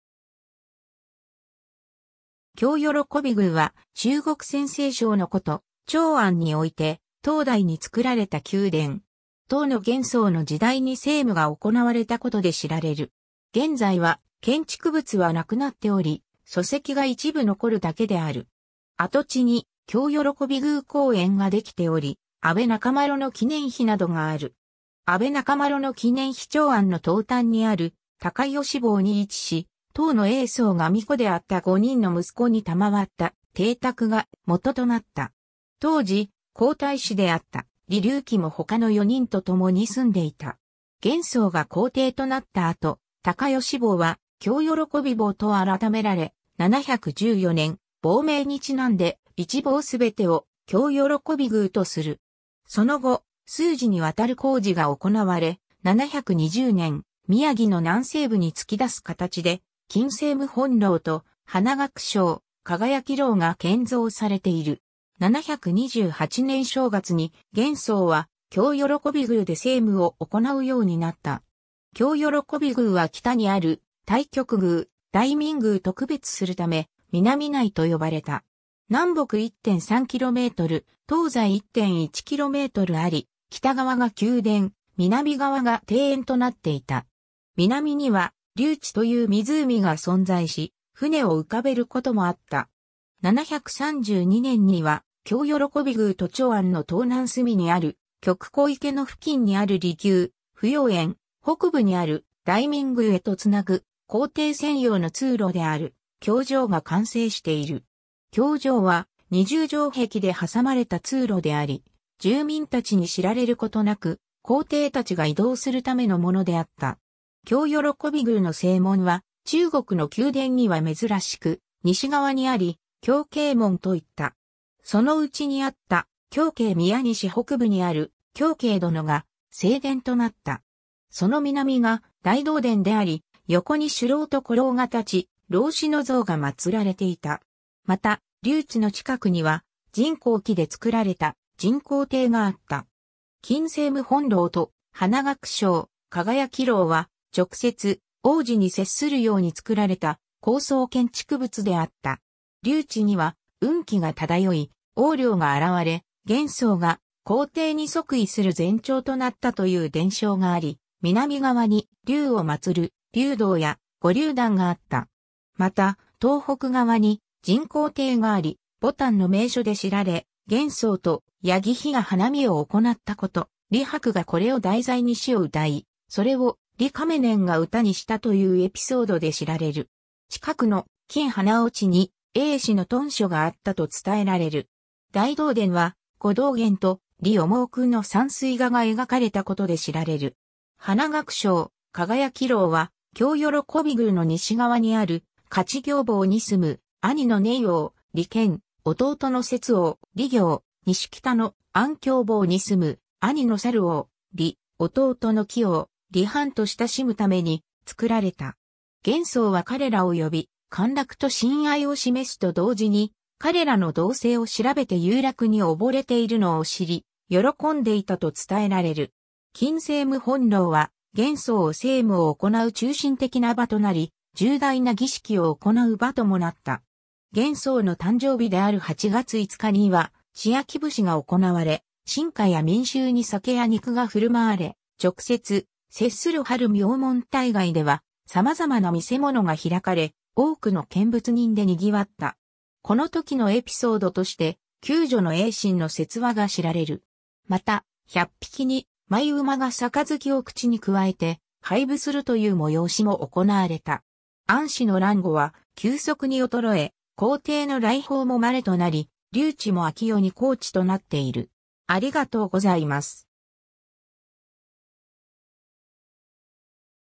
興慶宮
興慶宮（こうけいきゅう）は、中国陝西省の古都、長安（西安市）において、唐代に造られた宮殿。
唐の玄宗の時代に政務が行われたことで知られる。
 現在は、建築物はなくなっており、礎石が一部残るだけである。
跡地に「興慶宮公園」ができており、阿倍仲麻呂の記念碑などがある。
 阿倍仲麻呂の記念碑 長安の東端にある「隆慶坊」に位置し、唐の睿宗が皇子であった五人の息子に賜った邸宅が元となった。
当時、皇太子であった李隆基（後の玄宗）も他の四人とともに住んでいた。
 玄宗が皇帝となった後、「隆慶坊」は、「興慶坊」と改められ、714年（開元2年）、坊名にちなんで、一坊全てを「興慶宮」とする。
その後、数次に渡る工事が行われ、720年（開元8年）、宮城の南西部に突き出す形で「勤政務本楼」と「花萼相輝楼」が建造されている。
728年（開元16年）正月に、玄宗は興慶宮で政務を行うようになった。
興慶宮は北にある「太極宮」、「大明宮」と区別するため、「南内」と呼ばれた。
南北1.3キロメートル、東西1.1キロメートルあり、北側が宮殿、南側が庭園となっていた。
南には、「竜池」という湖が存在し、船を浮かべることもあった。
732年（開元20年）には、興慶宮と長安の東南隅にある曲江池の付近にある離宮「芙蓉園」、北部にある「大明宮」へとつなぐ皇帝専用の通路である「夾城」が完成している。
「夾城」は、二重城壁で挟まれた通路であり、住民たちに知られることなく、皇帝たちが移動するためのものであった。
 興慶宮の正門は中国の宮殿には珍しく西側にあり、「興慶門」といった。
その内にあった興慶宮西北部にある「興慶殿」が正殿となった。
その南が「大同殿」であり、横に鐘楼と鼓楼が立ち、老子の像が祀られていた。
また、「竜池」の近くには、沈香木で作られた「沈香亭」があった。
「勤政務本楼」と「花萼相輝楼」は、直接、大路に接するようにつくられた高層建築物であった。
 「竜池」には、雲気がただよい、黄竜が現れ、玄宗が皇帝に即位する前兆となったという伝承があり、南側に、竜を祀る「竜堂」や「五竜壇」があった。
また、東北側に「沈香亭」があり、牡丹の名所で知られ、玄宗と楊貴妃が花見を行ったこと、李白がこれを題材に詩を詠い、それを李亀年が歌にしたというエピソードで知られる。
近くの「金花落」に衛士の屯所があったと伝えられる。
 「大同殿」は、呉道玄と李思訓の山水画が描かれたことで知られる。
 「花萼相輝楼」は、興慶宮の西側にある「勝業坊」に住む兄の寧王・李憲、弟の薛王・李業、西北の「安興坊」に住む兄の申王・李撝、弟の岐王・李範と親しむために、造られた（玄宗をいれるこの五人で「五王」と呼ばれていた）。
玄宗は彼らを呼び、歓楽と親愛を示すと同時に、彼らの動静を調べて遊楽に溺れているのを知り、喜んでいたと伝えられる。
 「勤政務本楼」は、玄宗を政務を行う中心的な場となり、重大な儀式を行う場ともなった。
玄宗の誕生日である8月5日には、千秋節が行われ、臣下や民衆に酒や肉がふるまわれ、直接、接する春明門大街では、様々な見世物が開かれ、多くの見物人でにぎわった。
この時のエピソードとして、宮女の永新の説話が知られる。
また、100匹に舞馬が杯を口にくわえて、拝舞するという催しも行われた。
 安史の乱後は、急速に衰え、皇帝の来訪もまれとなり、「竜池」も明代に耕地となっている。